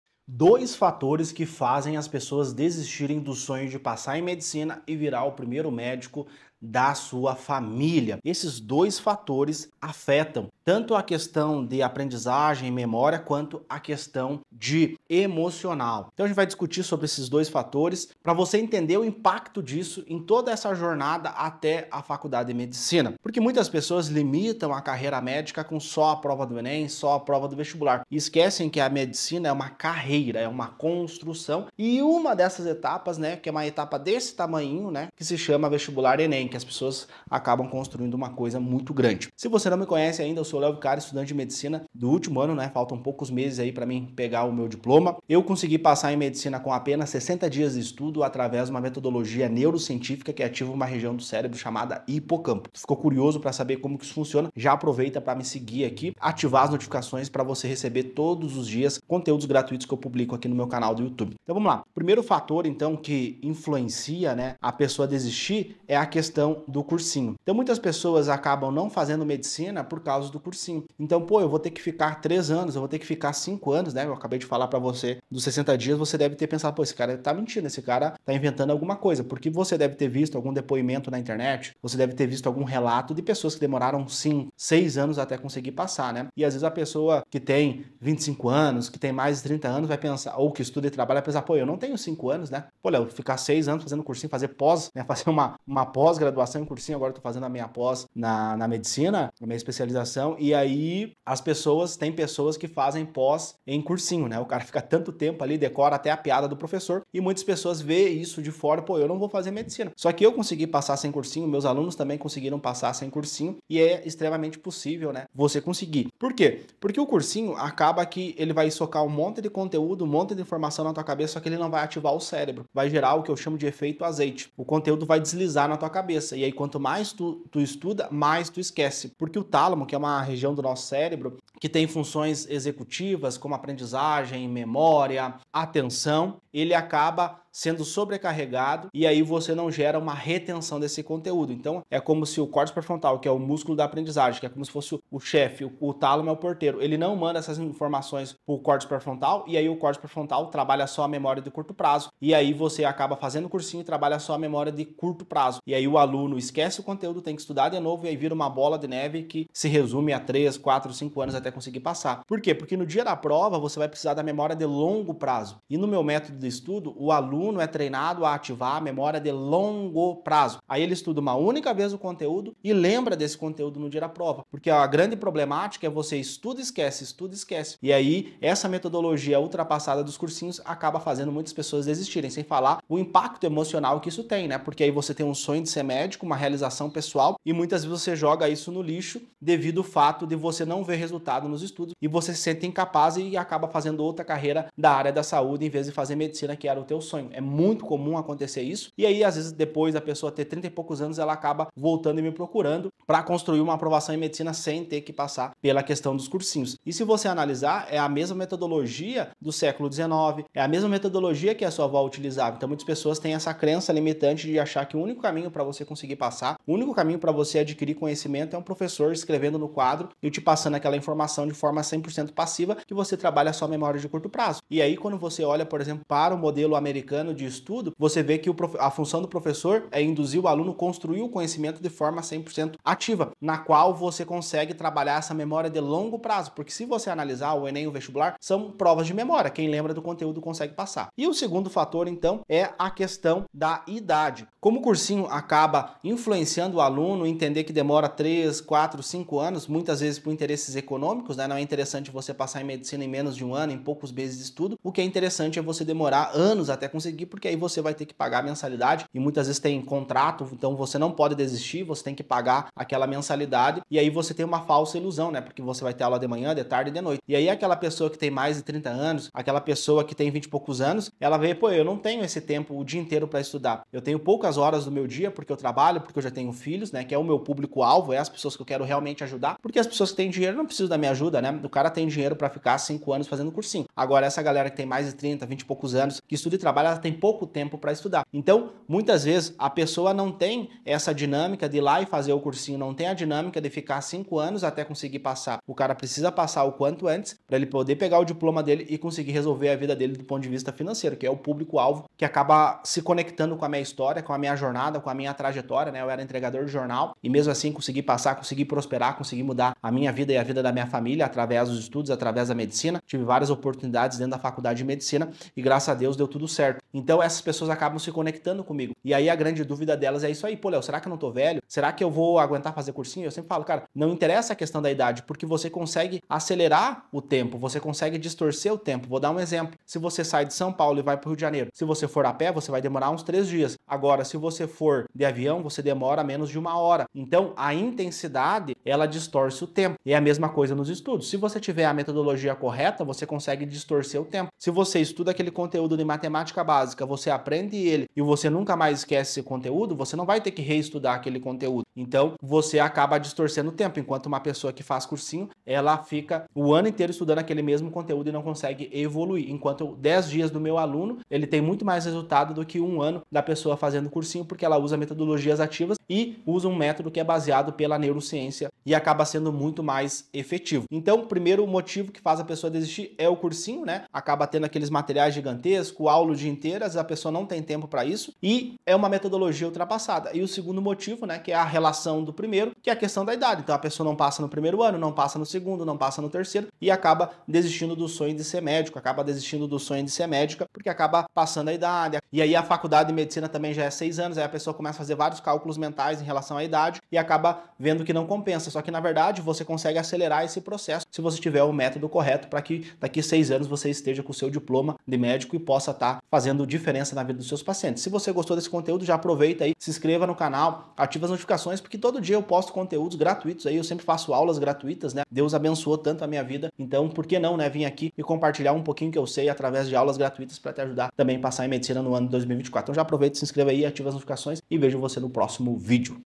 Por que Tantas Pessoas Desistem da Medicina? Conheça os 2 Motivos Decisivos!
Você já se perguntou por que tantos estudantes abandonam o sonho de se tornar médico, ou até mesmo já pensou em desistir? Então este vídeo é para você!

Nele vamos explorar 2 motivos decisivos na desistência e como superá-los.

E se você deseja passar em medicina, participe do meu curso gratuito, Desafio Você na Medicina, para aprender como passar em medicina em 60 dias. Serão 4 aulas gratuitas e online nos dias 29/01, 31/01, 02/02, e 04/02.